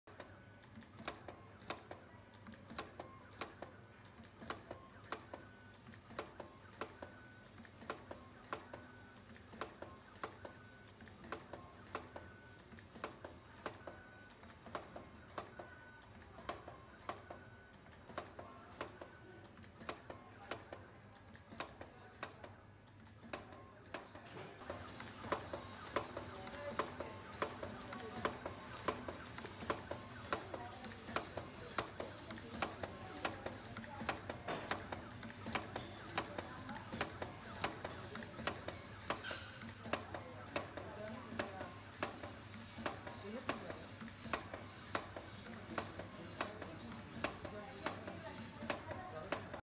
Automatic Hole Punching Webbing Cutting Machine
Automatic Angle WebbingCuttingMachine with Hole Punching
FOXSEW FX-150HX